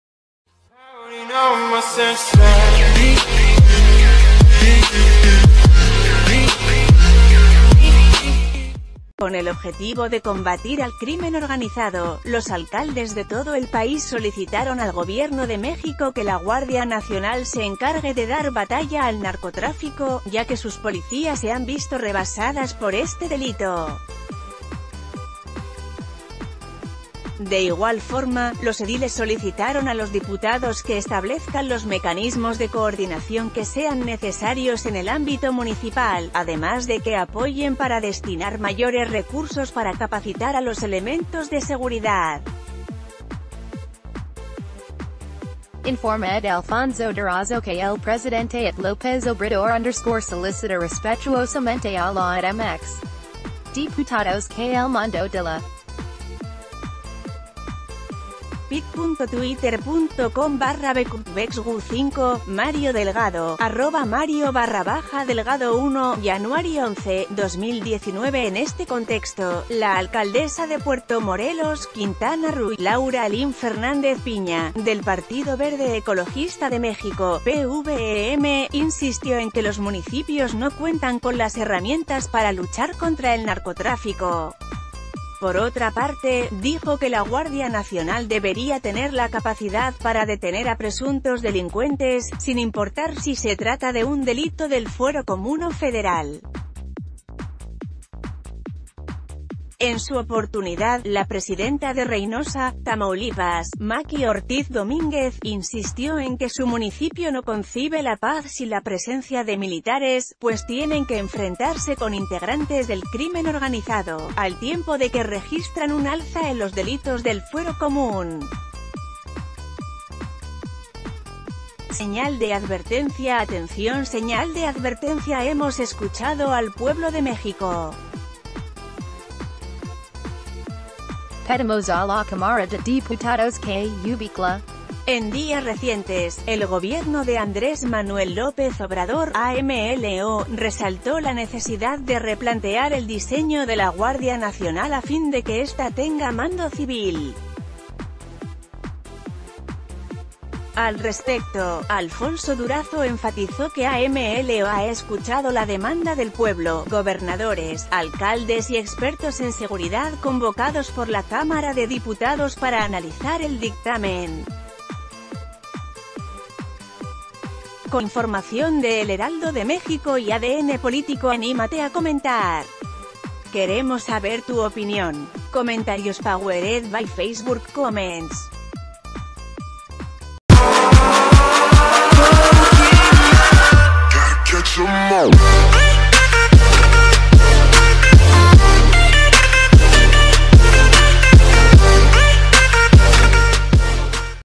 Alcaldes piden que Guardia Nacional combata al narcotráfico
Con el objetivo de combatir al crimen organizado, los alcaldes de todo el país solicitaron al Gobierno de México que la Guardia Nacional se encargue de dar batalla al narcotráfico, ya que sus policías se han visto rebasadas por este delito. De igual forma, los ediles solicitaron a los diputados que ...